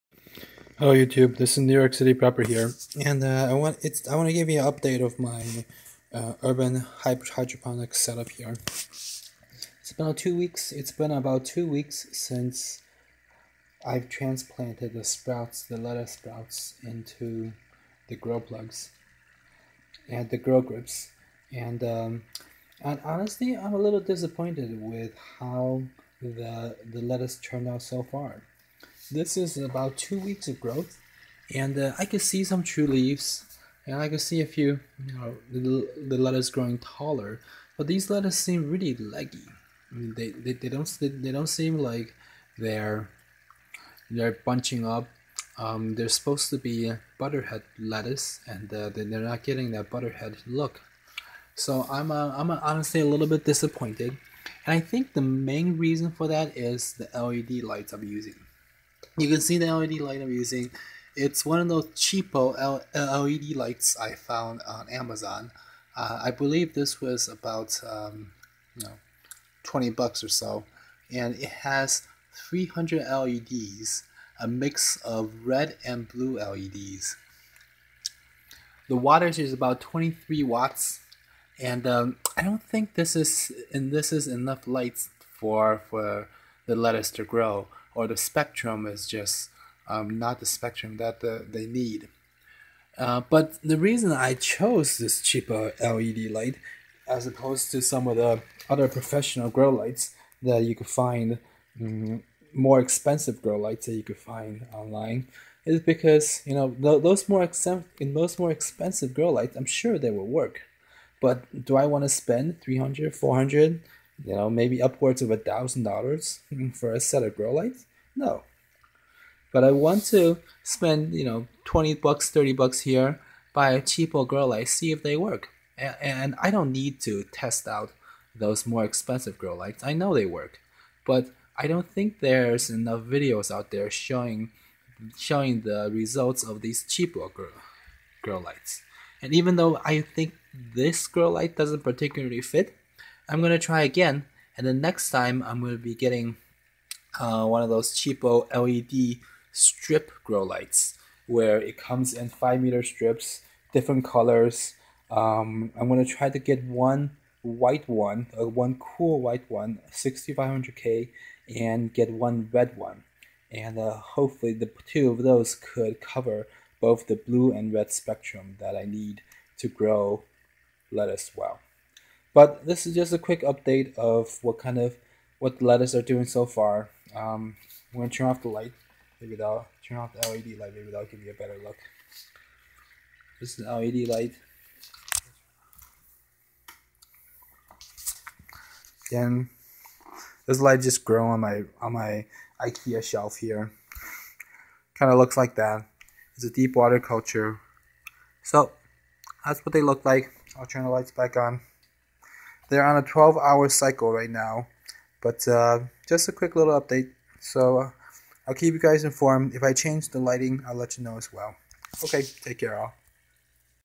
Urban Hydroponics with cheap LED grow light 2
I recently set up a Deep Water Culture hydroponic set in my apartment.   I mainly want to test out a cheap LED grow light from Amazon.  It is a 14 Watt 225 LED Blue + Red Indoor Garden Hydroponic Plant Grow Light Pane for about $20. I will be update with more videos later showing the lettuce growth.